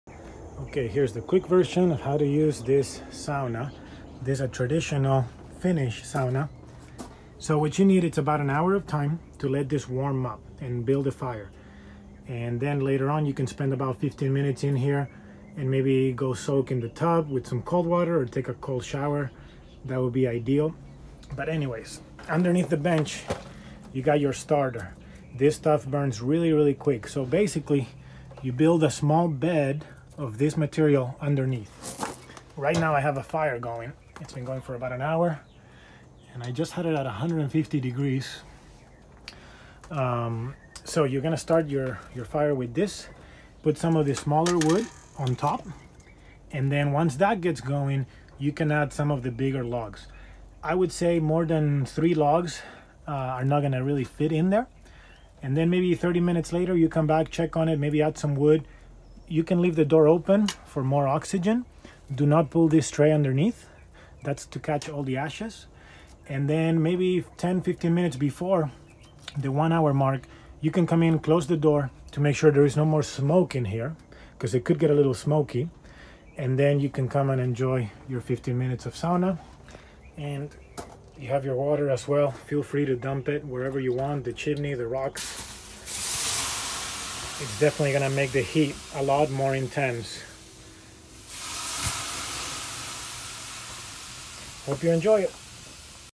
Quick How to Use our Finnish Wood Burning Sauna
This is a quick how to for our Airbnb guests & how to use the sauna.